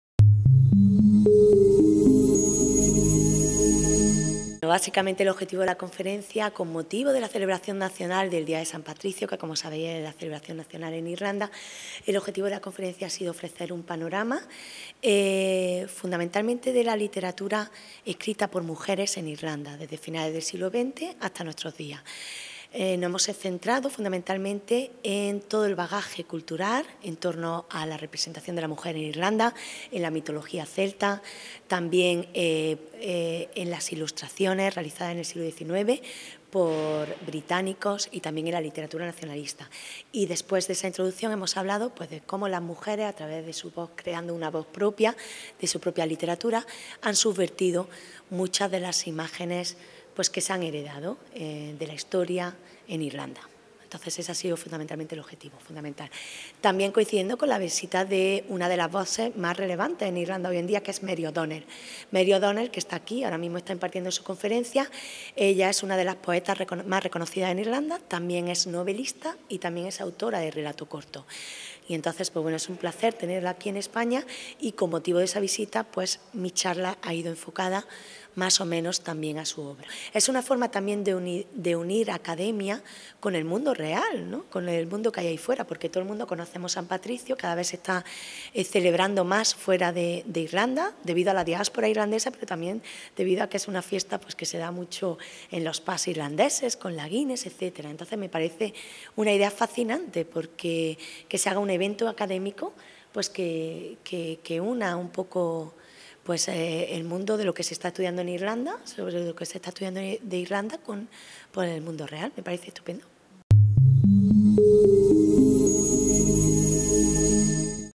La Facultad de Humanidades celebra la I Jornada de Estudios Irlandeses
La Facultad de Humanidades de la Universidad de Almería celebra este viernes 17 de marzo el Día de San Patricio, Fiesta Nacional de Irlanda, con un programa de actividades que ha girado en torno a las I Jornadas de Estudios Irlandeses.

La profesora titular de la Universidad de Granada, Pilar Villar Argáiz, ha pronunciado la conferencia ‘A New National Muse: Origins and Developments in Contemporary Writings by Irish Women Poets’ en la que ha hecho un recorrido por el panorama literario irlandés femenino desde finales del siglo XX hasta nuestros días “nos hemos centrado fundamentalmente en todo el bagaje cultural en torno a la representación de la mujer en Irlanda, en la mitología celta, también en las ilustraciones realizadas en el siglo XIX por británicos y también en la literatura nacionalista. Además, hemos hablado de cómo las mujeres han creado una voz propia a través de la Literatura y han subvertido muchas de las imágenes que se han heredado de la historia en Irlanda”.

Una de las voces literarias más relevantes de Irlanda, la escritora Mary O’Donnell, ha visitado la UAL para participar en los actos programados por el Día de San Patricio “ella es una de las poetas más reconocidas en Irlanda, es novelista y también autora de relato corto, es un placer tenerla aquí de visita y con motivo de ello mi charla ha ido también enfocada a su obra”, ha resaltado la profesora de la Universidad de Granada, Pilar Villar.

La escritora irlandesa Mary O’Donnell, prestigiosa poeta, novelista, autora de relatos cortos, ensayista y crítica de teatro, además de docente de escritura creativa en la Universidad de Galway, ha protagonizado la Lectura Literaria.

O’Donnell ha publicado cerca de una veintena de obras, entre las que destaca The Light-Makers, considerada la mejor novela irlandesa del año 1992 por The Sunday Tribune. Asimismo, ha sido galardonada con numerosos premios como The William Aillingham Award, The Listowel Writers’ Week Award o el Hennessy Award.